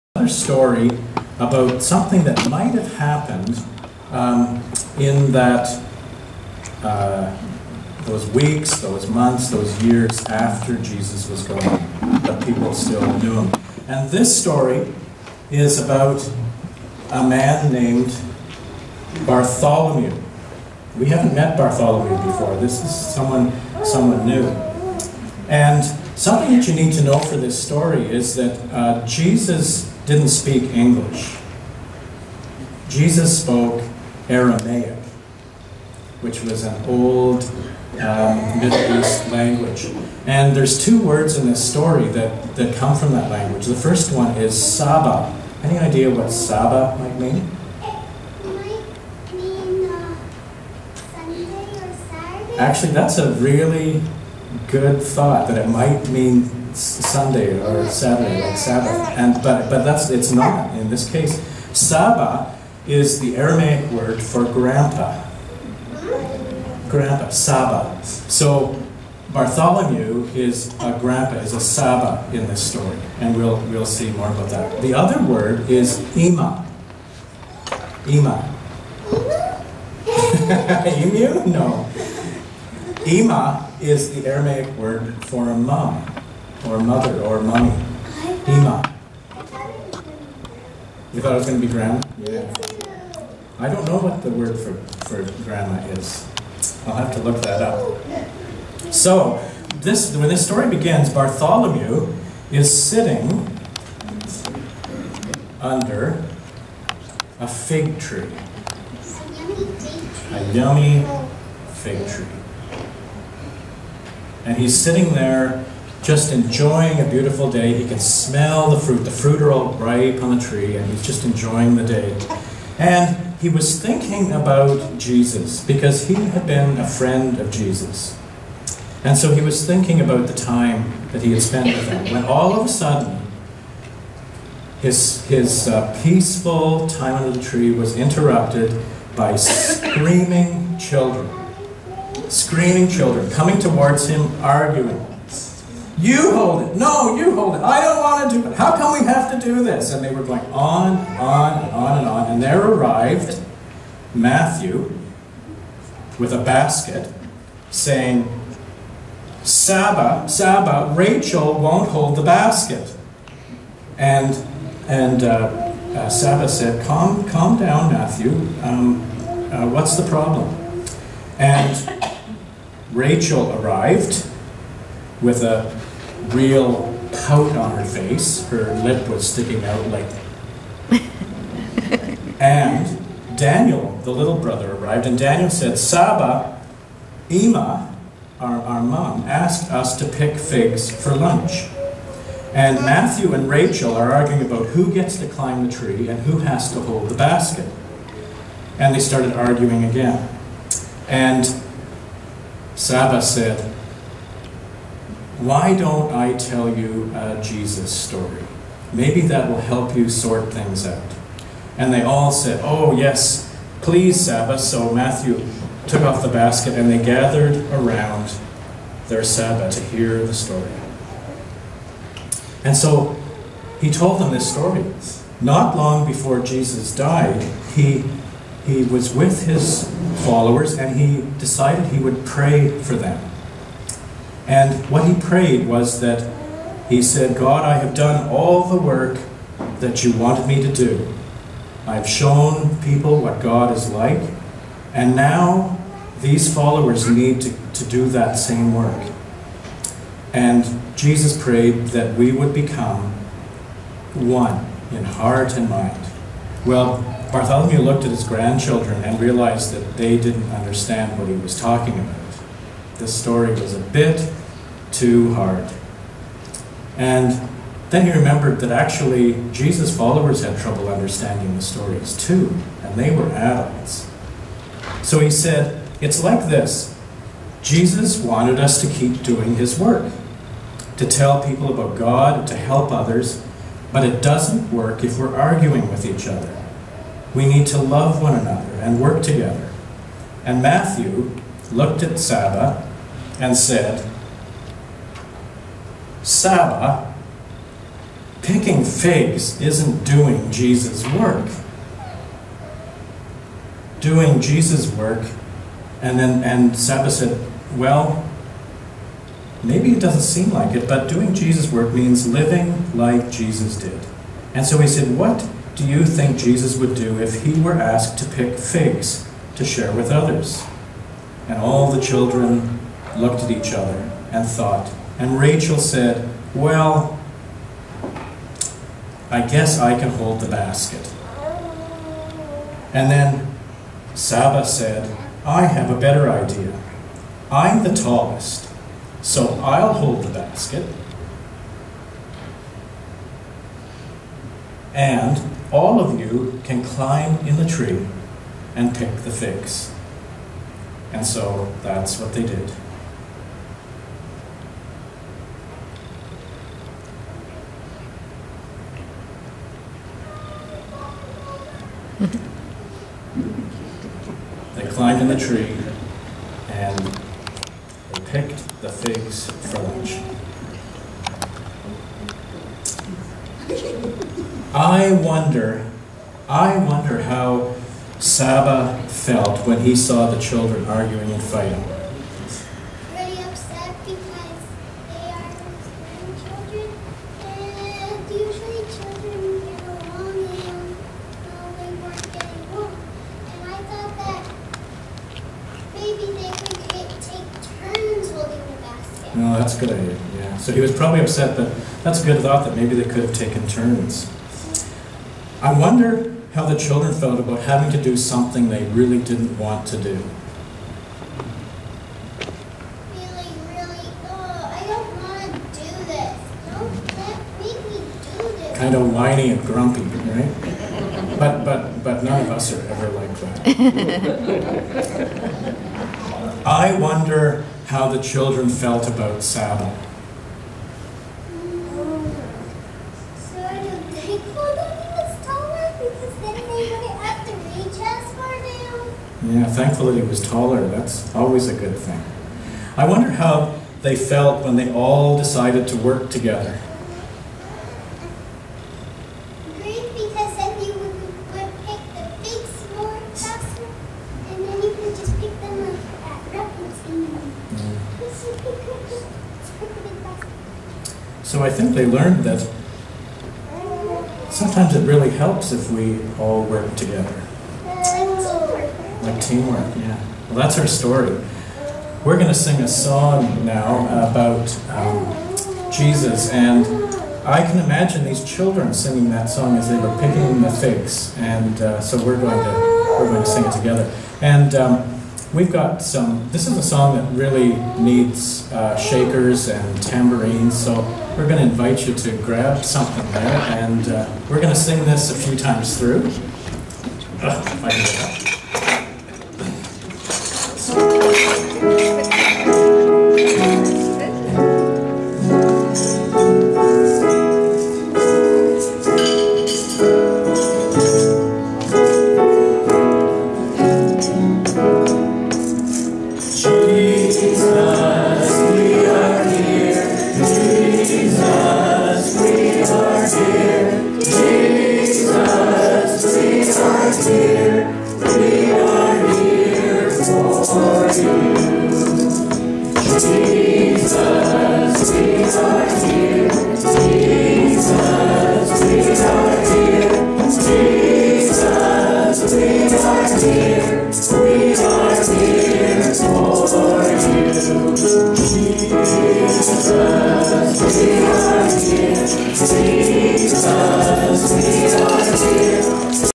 June 1, 2014 Word for All Ages: "Working Together"
~ based on John 17:1-11 from Seasons of the Spirit
told by Jim Hannah